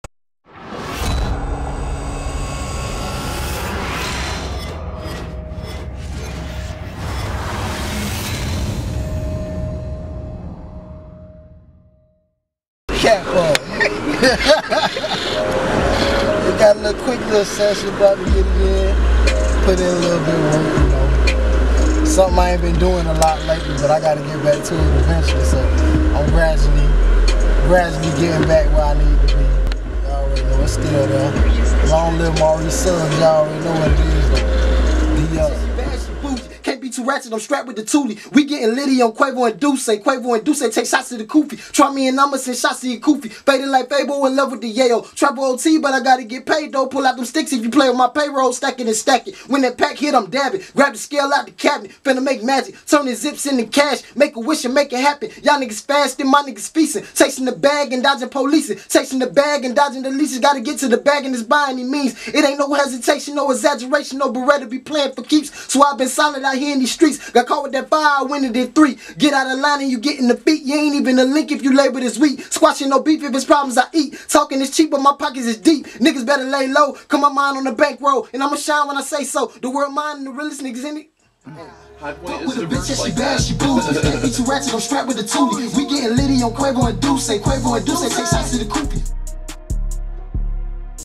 Beretta Tha G (studio vlog)
sneak peak @Beretta The G cooking new music in the studio, new music on the way, stay locked  in!